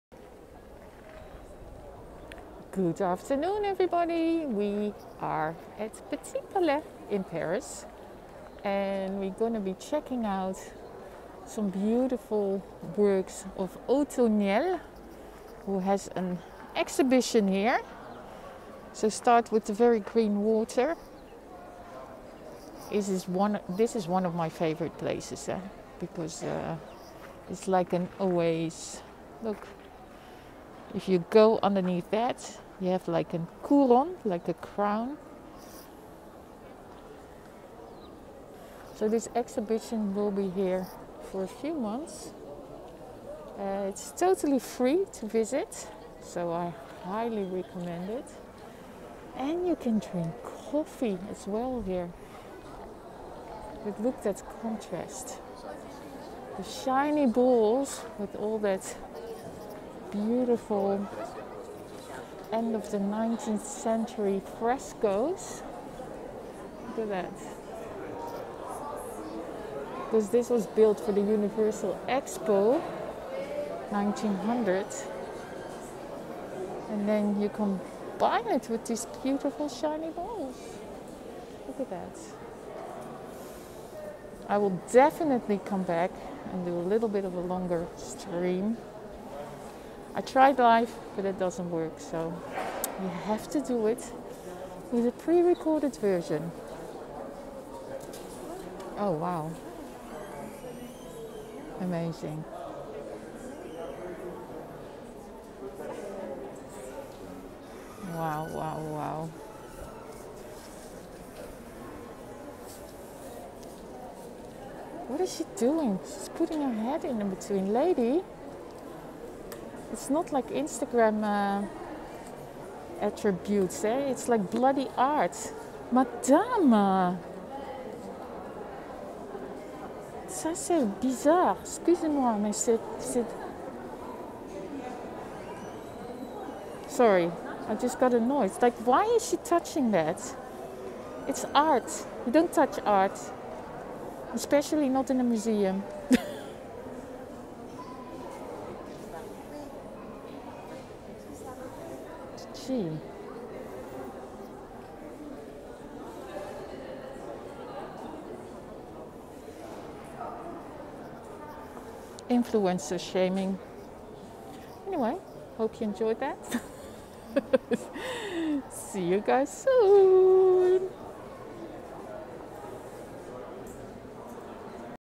Shiny balls by Othoniel in Le Petit Palais in Paris
Not to be missed! Jean-Michel Othoniel in Le Petit Palais in Paris. Lots of shiny balls in his art. Make sure to visit Othoniel in Paris, it's totally free entrance.